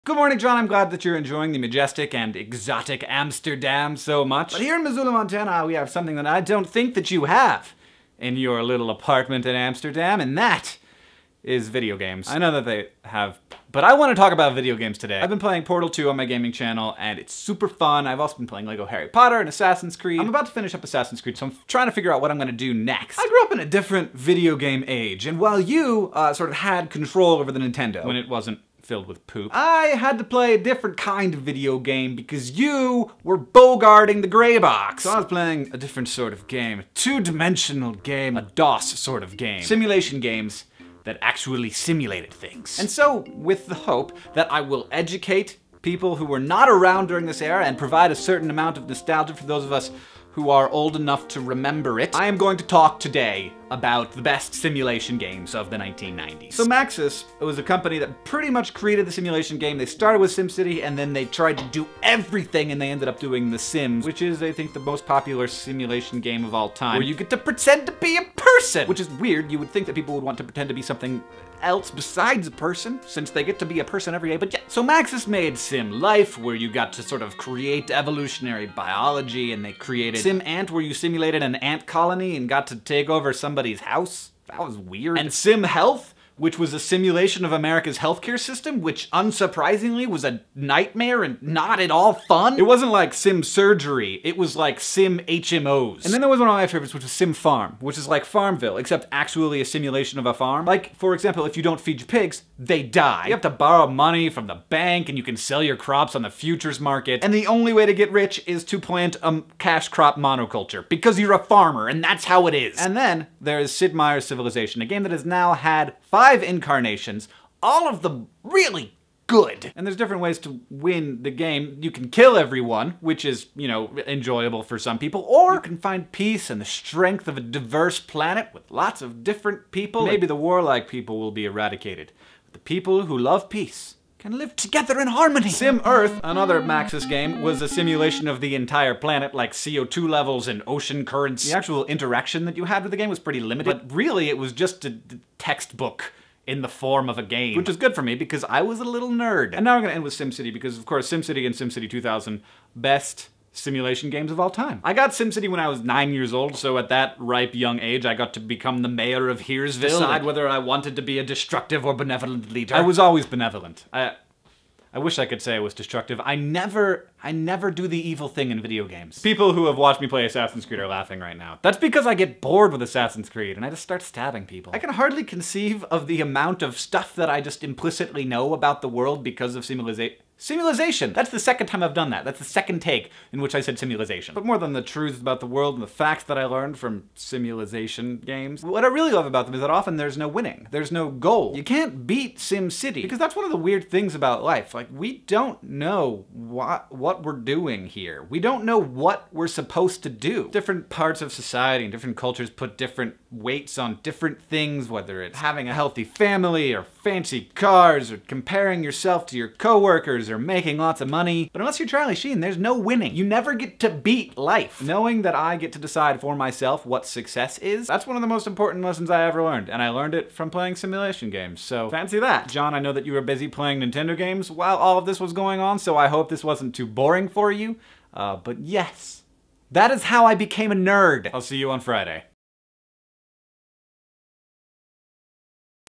1990's Sim Games. SO GOOD!
In which Hank talks about the lessons he learned growing up playing simulation games (and does a very good job of not complaining about how mindless gaming has become.)

Sim Games taught me a huge amount about my world and also about who I am. From SImCity I learned that as much as a third of a city is taken up by its roads. From SimEarth I learned what that crustaceans existed before fish. From SimTower I learned that things are never as simple as they seem. From Civilizaiton I learned that peace is orders of magnitude more difficult and less glamorous than war. From Tycoon games I learned the value of opportunity and also how to be careful not to squander the value you have.

It seems a little silly, but there it is. You can get a lot of these games for free (all you need is DOS BOX and a good google search if you'd like to try them out.